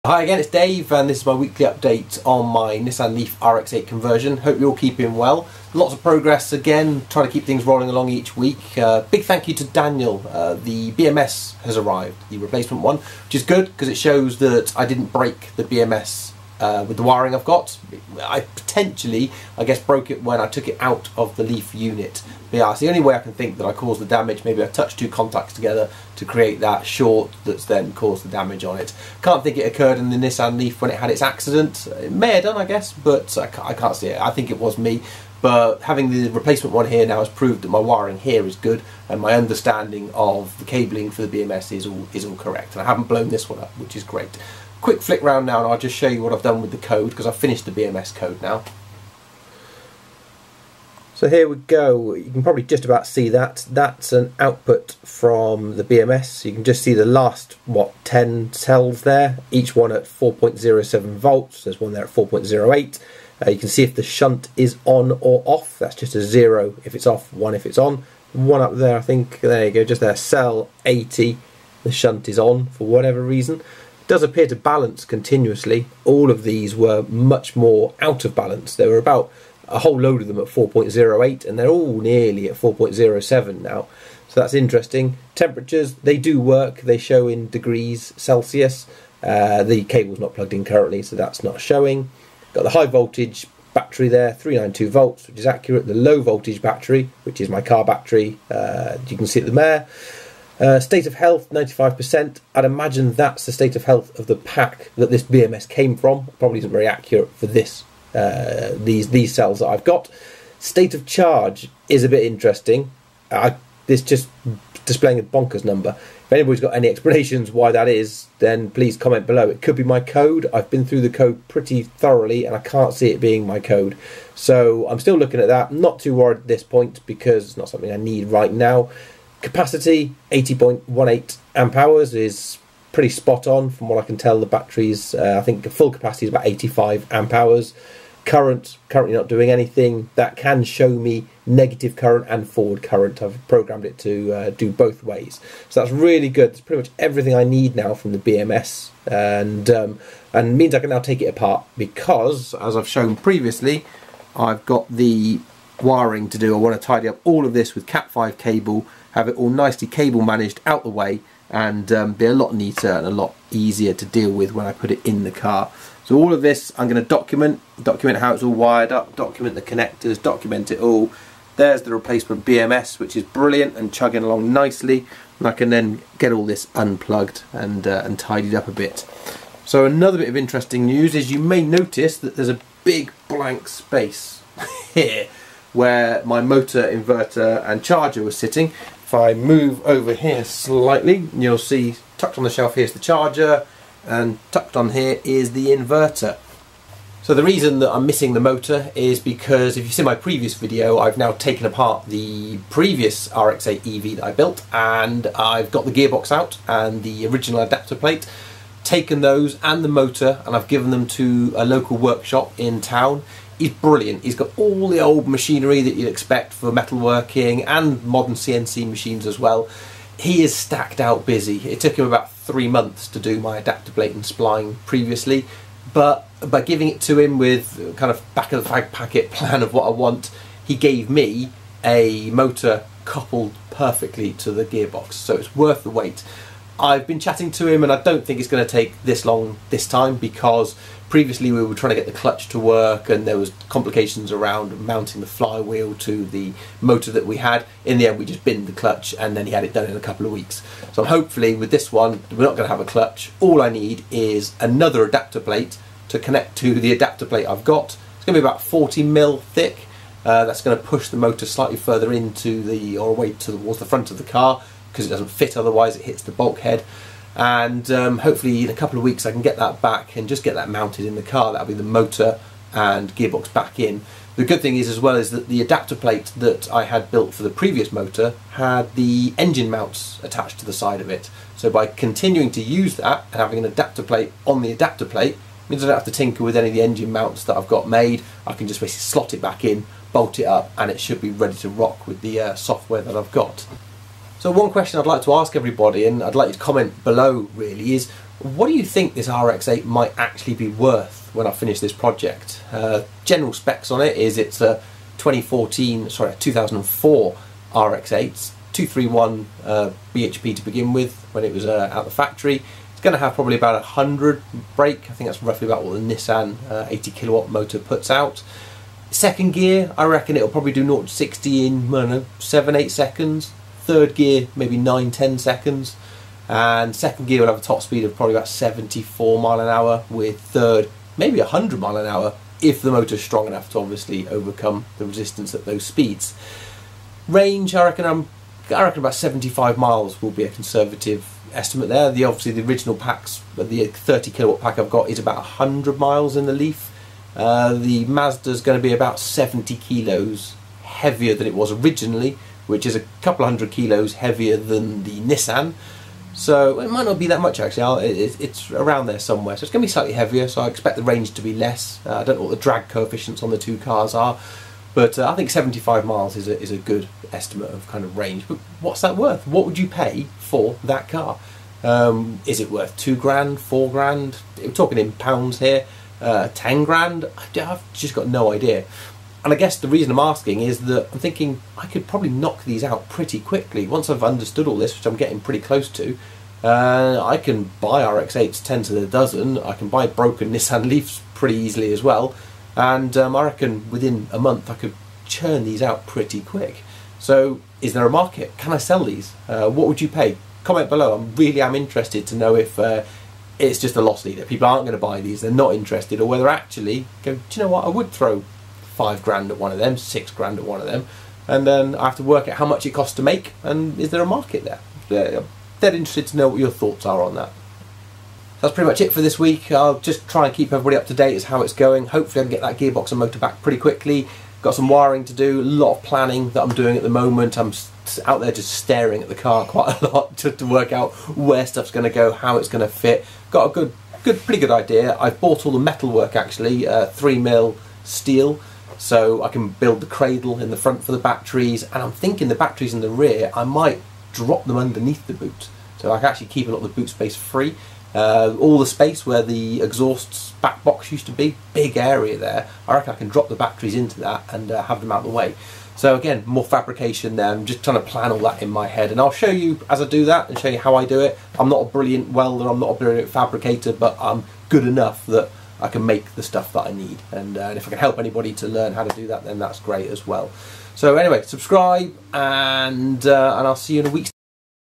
Nissan Leaf to RX8 Conversion, Week 9
I have shipped the motor and gearbox off to have the adaptor plate and spline made up. Thanks Daniel Öster for the replacement BMS - this is working great and has allowed me to finish my BMS code.

Lots of planning going on, next starts the fabrication process for the various parts...

Various items for sale on Ebay,
AC motor & inverter kit

Dc-Dc converter

Nissan Leaf Gearbox

Check out Daniel's channel below.